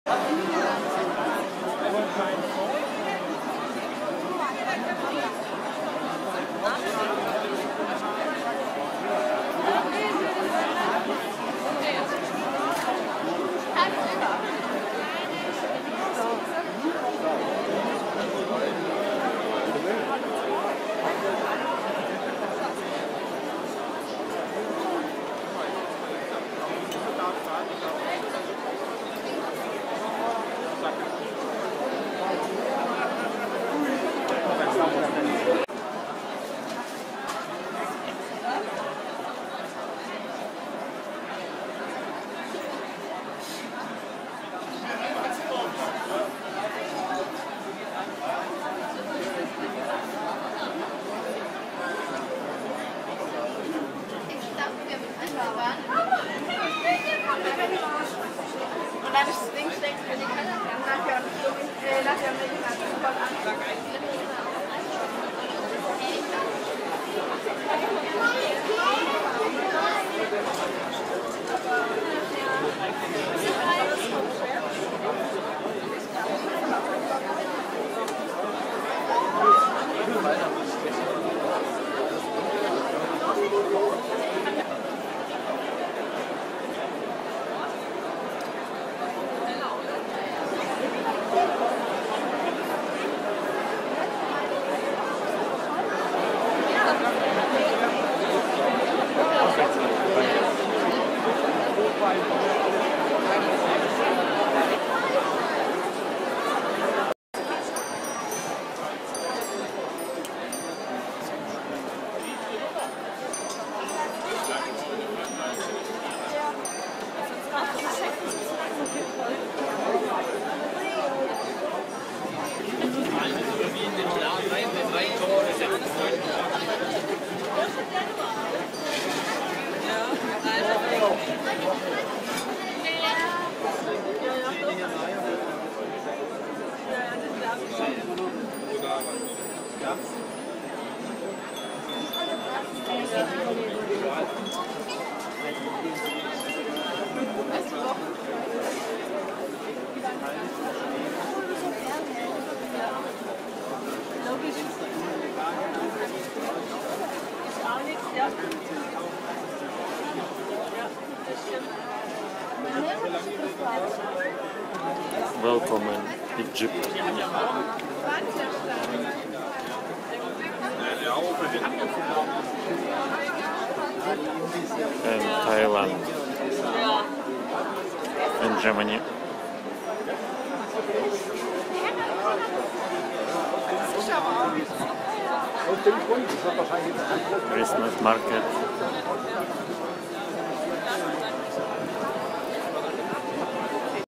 Germany Stuttgart city centre Rathaus Marktplatz. Штутгарт центр Горсовет
3840x2160p Германия Штутгарт центр Горсовет Рыночная пл. Рождественская ярмарка в декабре 2015.
Weihnachten in Deutschland
Christmas market